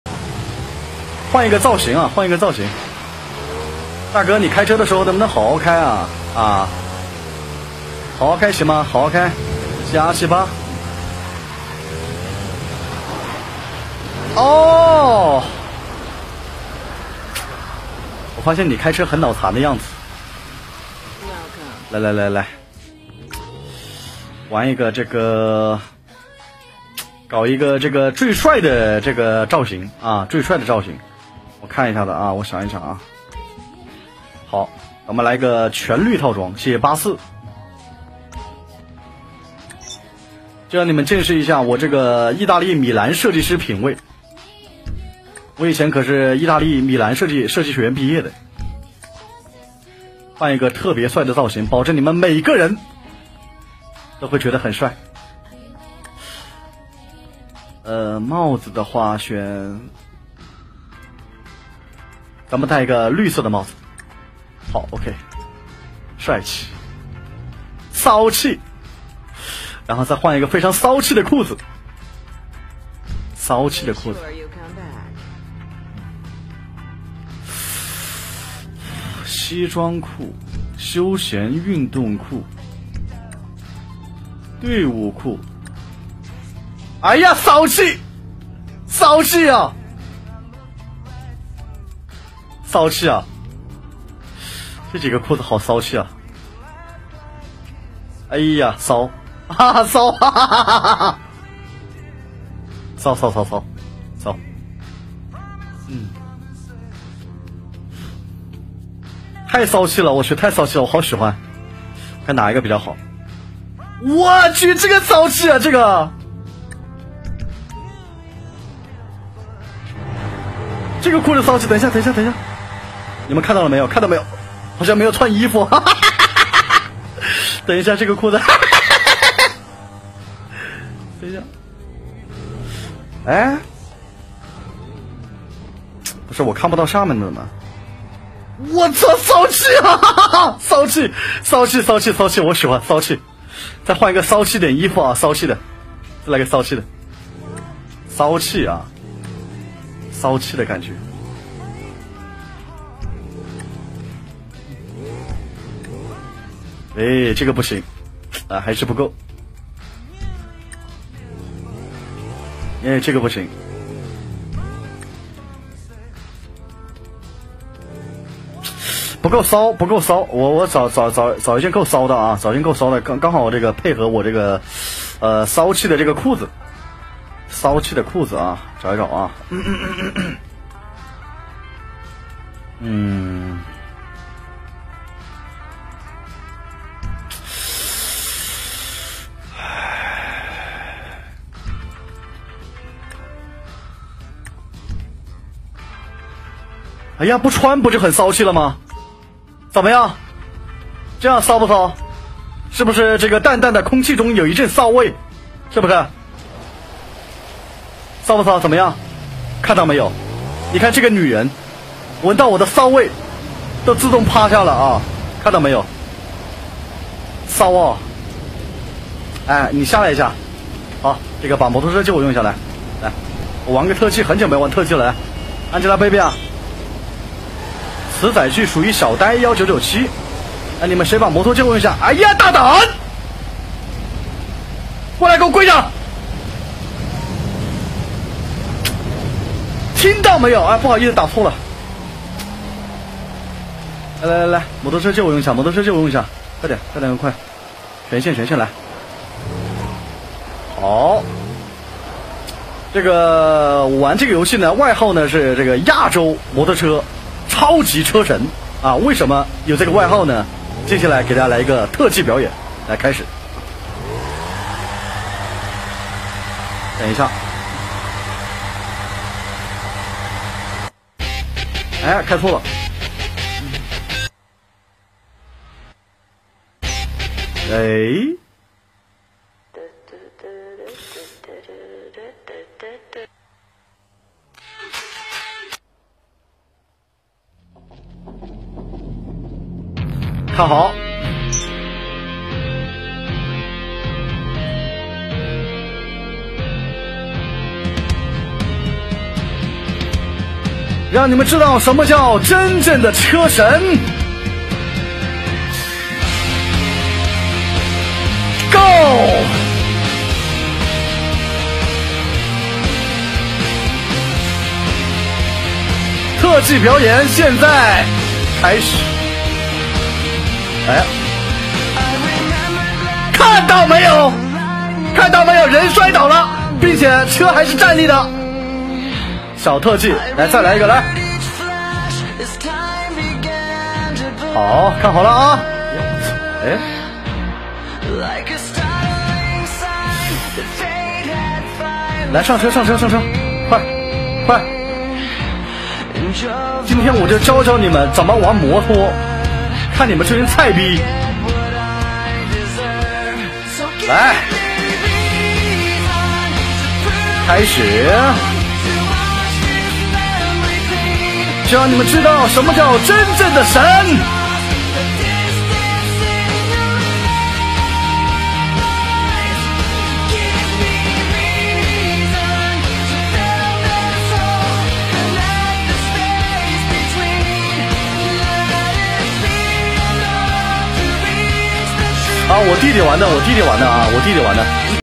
楚河换了一身骚气的服装，依然没有玩好摩托特技
【9月12日】楚河换了一身骚气的服装，依然没有玩好摩托特技